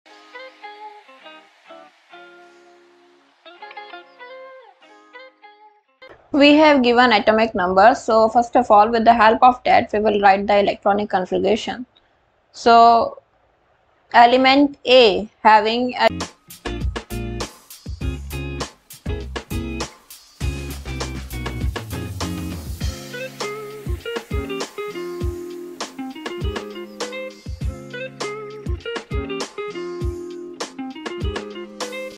Two elements, A and B have atomic number 9 and 11 respectively 1. What type of bond is formed? 2. S…
Two elements, A and B have atomic number 9 and 11 respectively 1. What type of bond is formed? 2. Show how it is formed 3. State two properties of the compound formed